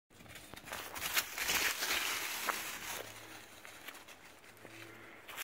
Seamus running
A very short clip of my boy on a trail.